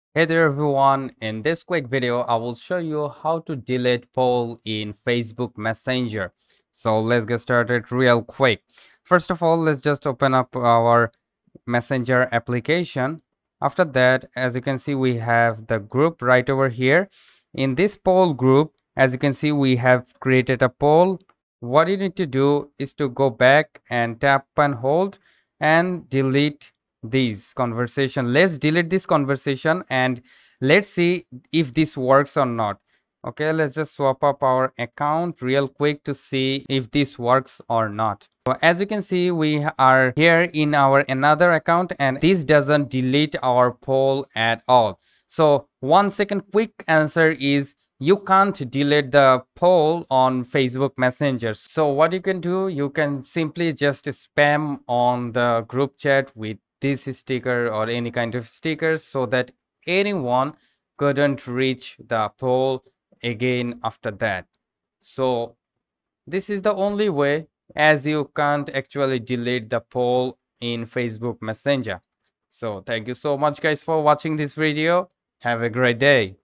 How to delete poll in messenger (2025)
Hey Everyone, Welcome to Tutorial Arena! In this video, I'm going to Show you how to delete poll in messenger after new updates. it will be the latest technique to solve this issue to remove poll from conversation in messenger of facebook.